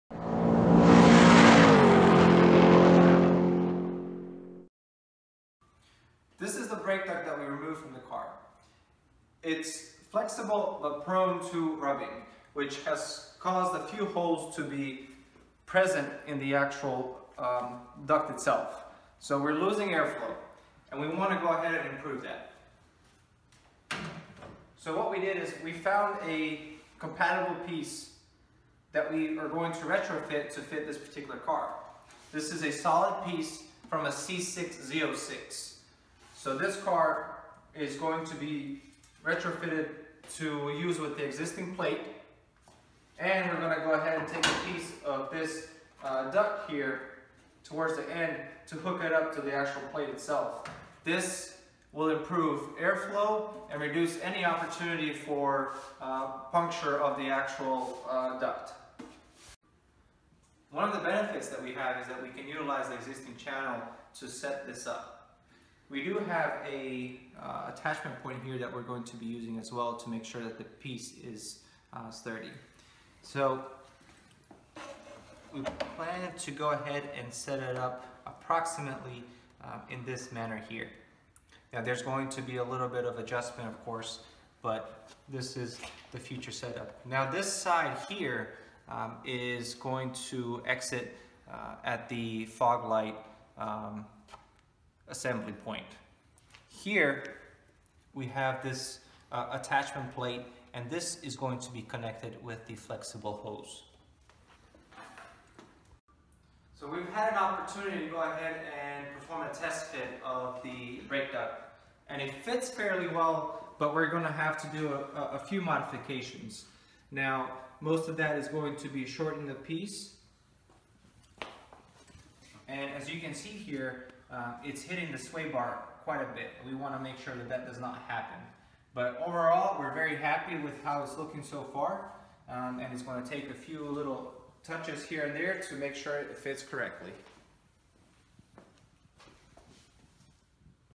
MX-5 Endurance Racer 6 - Z06 Brake Cooling Upgrade Test
We're upgrading the cooling for the front brakes with some Corvette Z06 parts